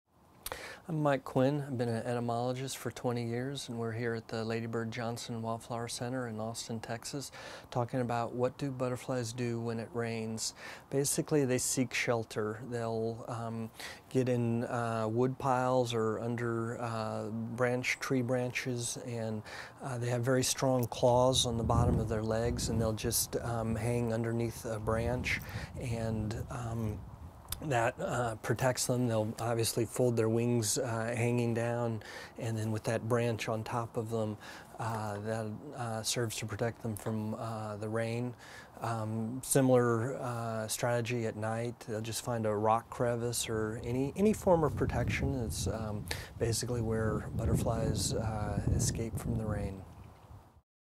Insect Information : What Do Butterflies Do When it Rains?
When it rains, butterflies seek shelter under tree branches, in a rock crevice or in a wood pile, often hanging from the strong claws on the bottom of their legs. Learn how butterflies will fold their wings down, to prevent getting wet in the rain, with information from an entomologist and insect specialist in this free video on butterflies.

Expert: Mike Quinn
Bio: Mike Quinn has a Master of Science degree in entomology from Texas A&M University.